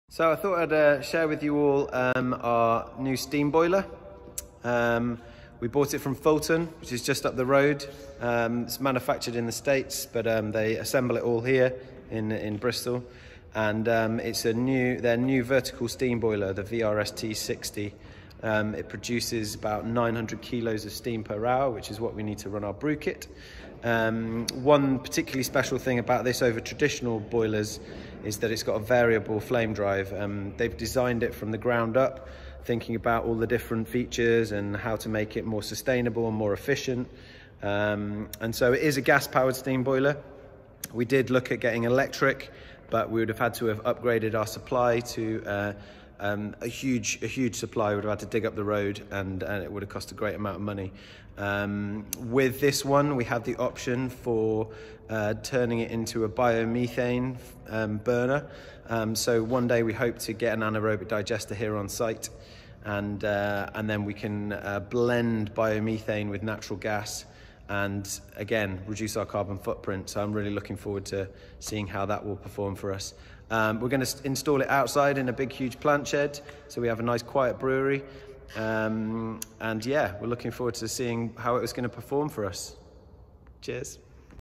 Building A Brewery | Behind the Scenes Part 2 | The Steam Boiler
Welcome back to Building A Brewery, our behind-the-scenes series into the making of Wiper and True's beautiful new brewery and taproom in Old Market, Bristol.

In this video we introduce you to the VRS T60: the colossal new steam boiler which powers the brewkit.